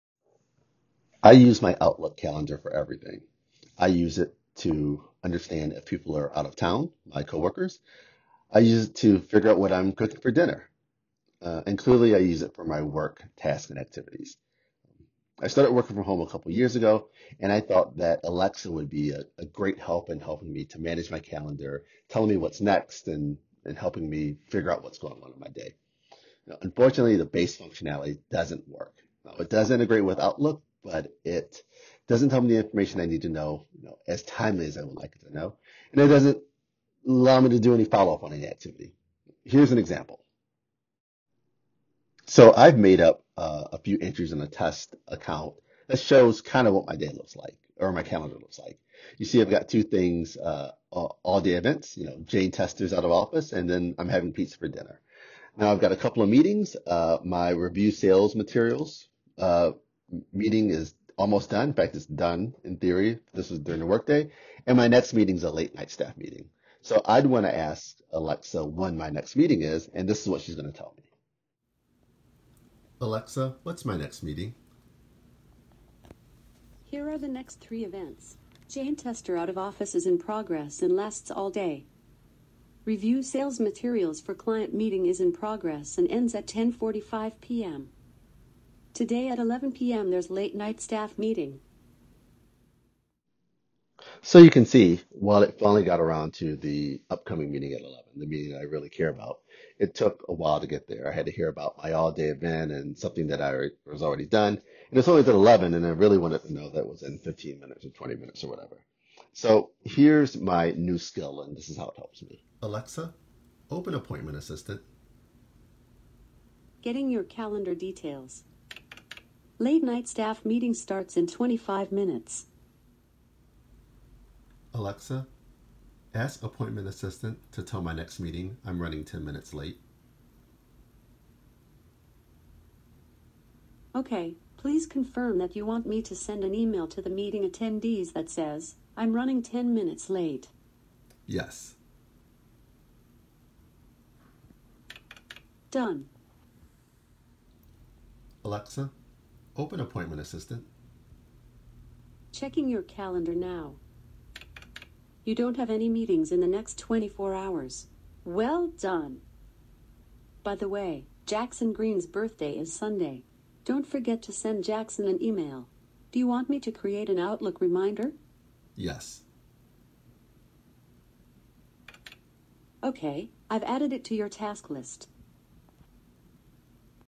Appointment Assistant
An
Alexa skill that integrates with Microsoft Outlook to support managing your upcoming meeting events. This is submitted as part of the Devpost-hosted Amazon Alexa Life Hacks Challenge